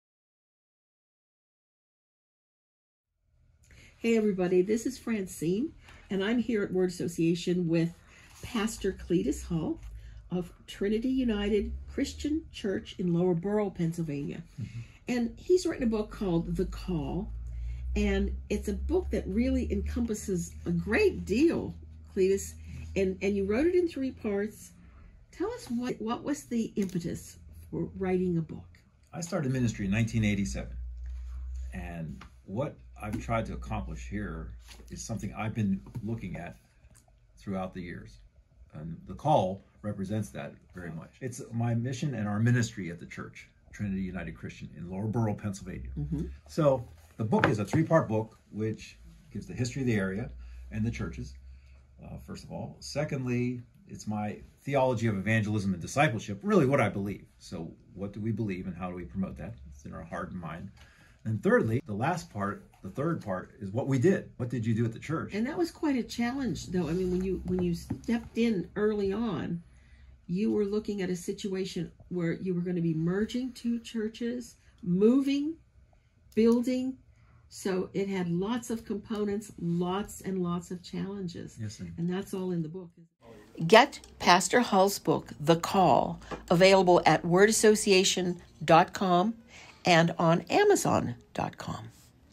The Call - Pastor Cletus Hull - What Was The Impetus For Writing The Book?
We sat down with Pastor Cletus Hull to discuss his book, The Call - a true story in three parts. Hull gives us a look into the rich history of the area and the church, and how he helped the merging of two parishes.